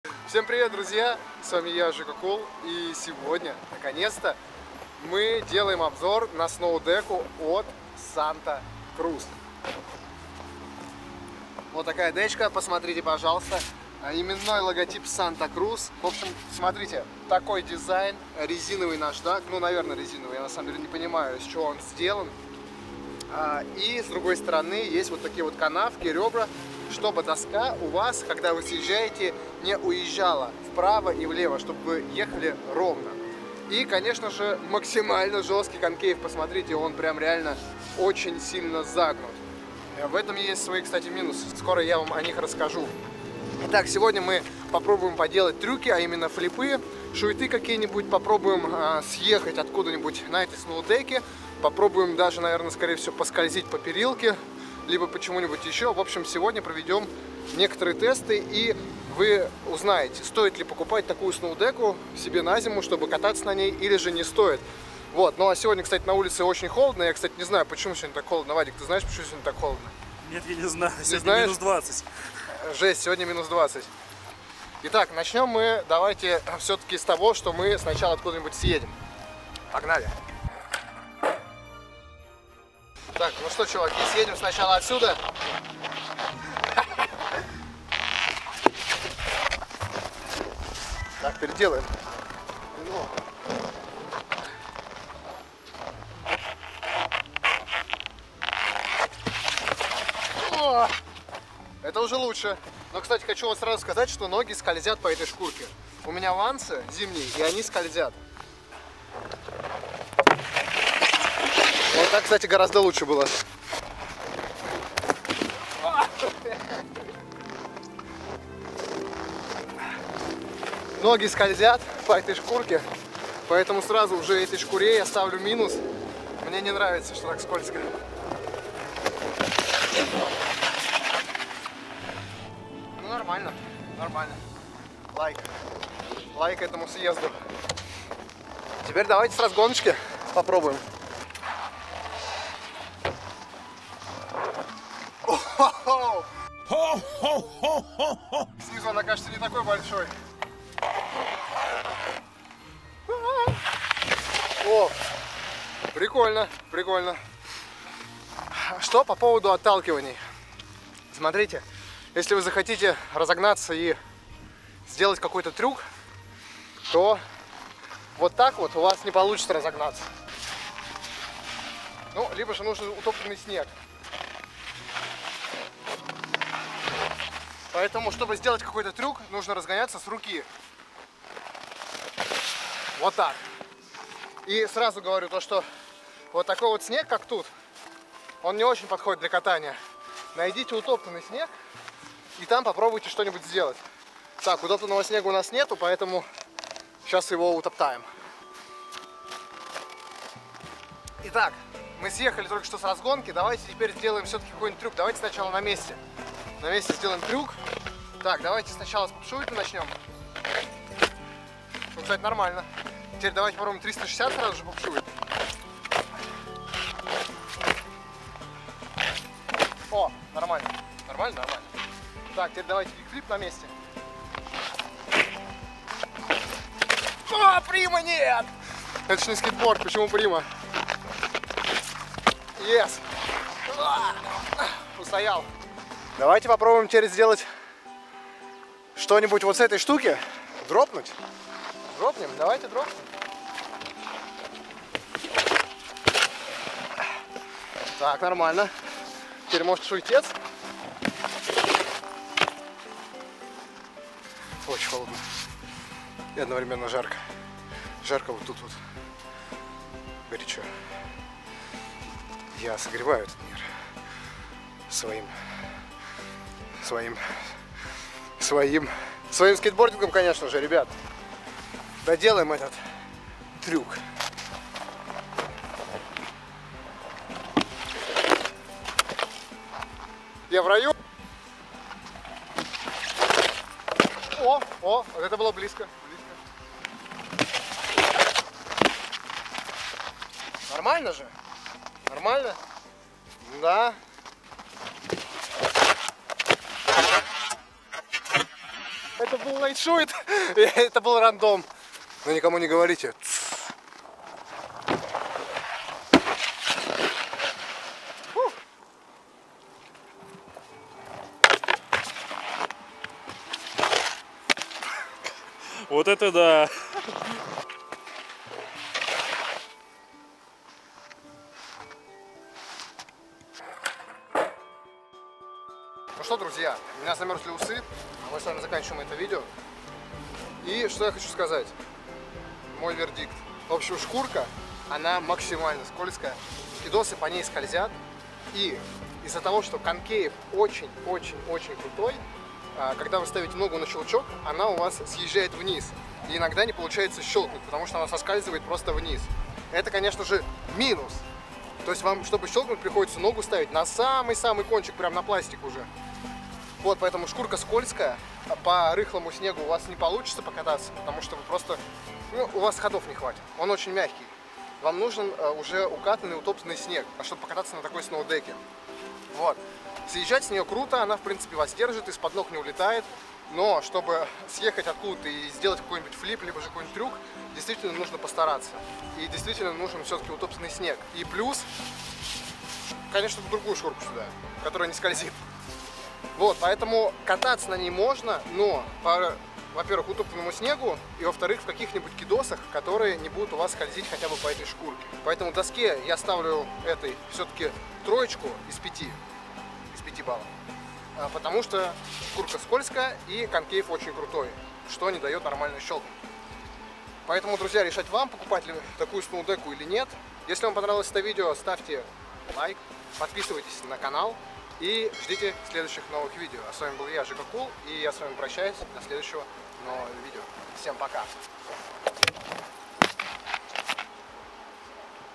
СНОУСКЕЙТ SANTA CRUZ ОБЗОР
В этом видео я буду тестировать сноускейт от SANTA CRUZ, проверим можно ли кататься на данном сноускейте и стоит ли его покупать!

✅Хочешь освоить скейтбординг с нуля? Мой видеогайд научит тебя всему, что нужно!